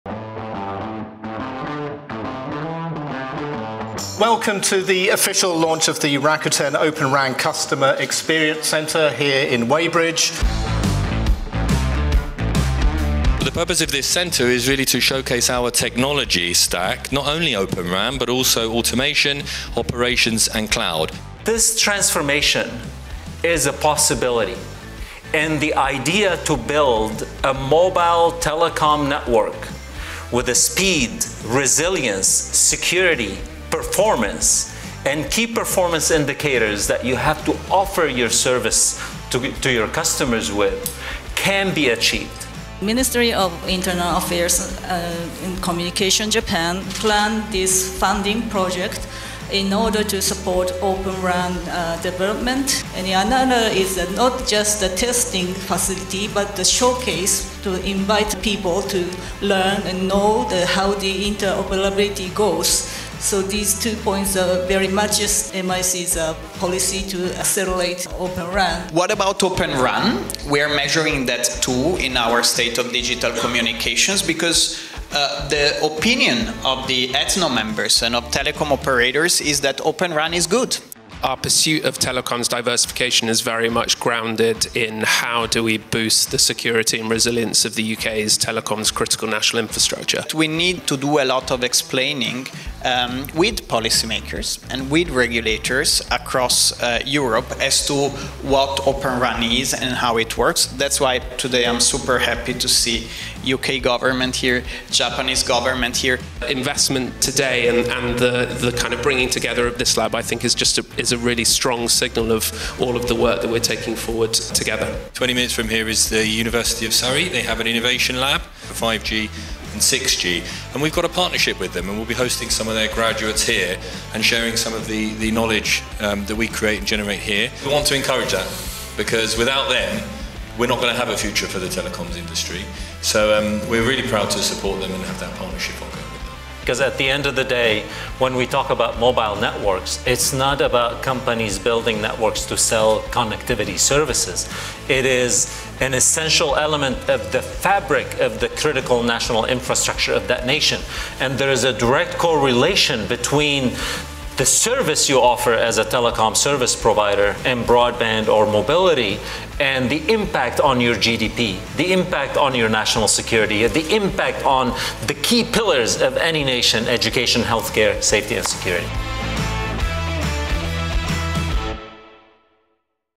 Welcome to the Rakuten Open RAN Customer Experience Center
At the official launch of the Rakuten Open RAN Customer Experience Center in Weybridge, UK, executives and representatives from Rakuten Symphony, the Japanese and UK governments and telecom industry body ETNO discuss the appeal and impact of open, cloud-enabled communications networks and explain how the new facility can help service providers meet digital transformation and sustainability goals.